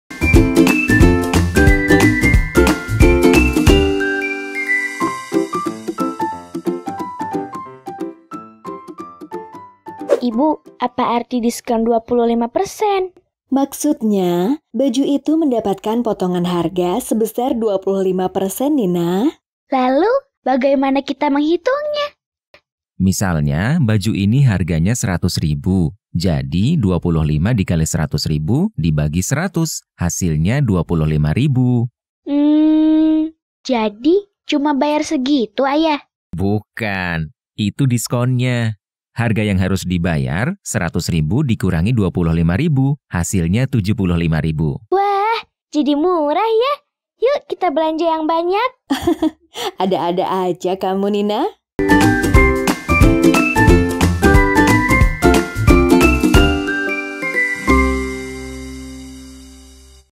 Menjelaskan tentang Persen - Bersama Erlangga (Video Animasi)
Hai Sahabat Erlangga!

Pernahkah kamu belajar tentang persen? Jika belum, yuk kita simak sama-sama apa itu persen dan cara menghitungnya dalam kehidupan sehari-hari melalui keseharian Nina dan keluarga.